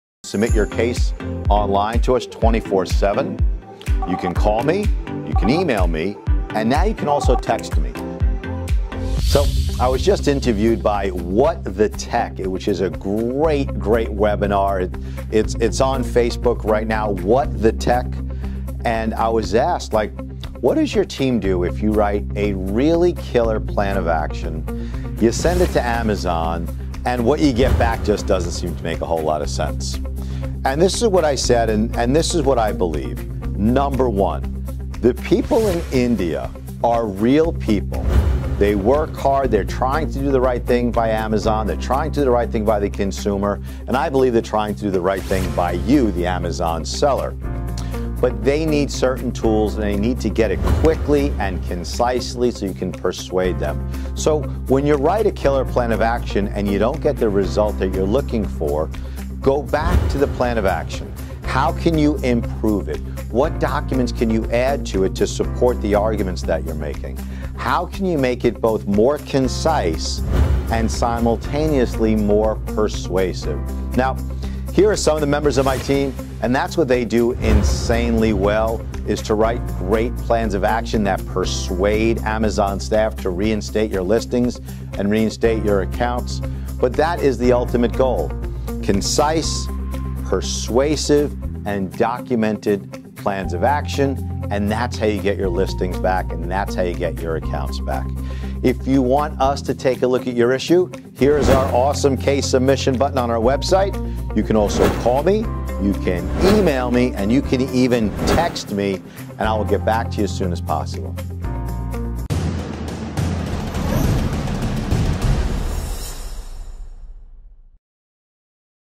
How to Persuade Amazon to Reinstate Your Selling Privileges w/ POA's
So I was just interviewed by What the Teck, which is a great, great webinar. It's on Facebook right now. What the Teck. And I was asked, "What does your team do if you write a really killer plan of action? You send it to Amazon, and what you get back just doesn't seem to make a whole lot of sense." And this is what I said, and this is what I believe. Number one, the people in India are real people. They work hard. They're trying to do the right thing by Amazon. They're trying to do the right thing by the consumer. And I believe they're trying to do the right thing by you, the Amazon seller. But they need certain tools and they need to get it quickly and concisely so you can persuade them.

So when you write a killer plan of action and you don't get the result that you're looking for, go back to the plan of action. How can you improve it? What documents can you add to it to support the arguments that you're making? How can you make it both more concise and simultaneously more persuasive? Now, here are some of the members of my team, and that's what they do insanely well, is to write great plans of action that persuade Amazon staff to reinstate your listings and reinstate your accounts. But that is the ultimate goal. Concise, persuasive, and documented plans of action. And that's how you get your listings back. And that's how you get your accounts back. If you want us to take a look at your issue, here is our awesome case submission button on our website.